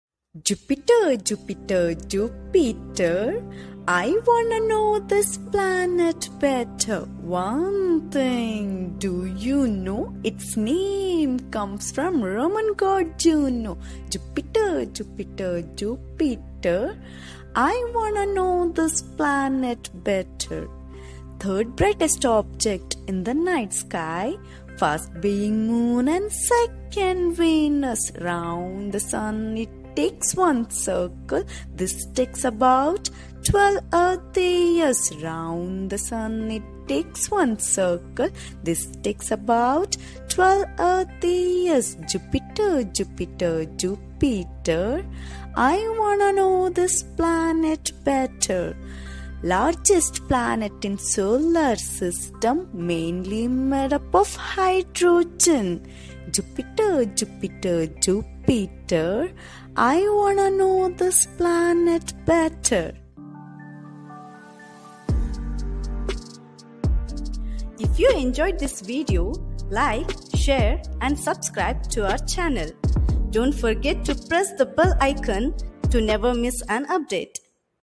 Poem on Jupiter for kids, penned by Dr. Alka Kinra
Catchy poem on Jupiter for kids# largest planet Jupiter# 3rd brighest object in the night sky
Way back in 2009, sitting in a waiting room of a clinic, the catchy poem was created for kids to know and learn few facts about mystery planet Jupiter.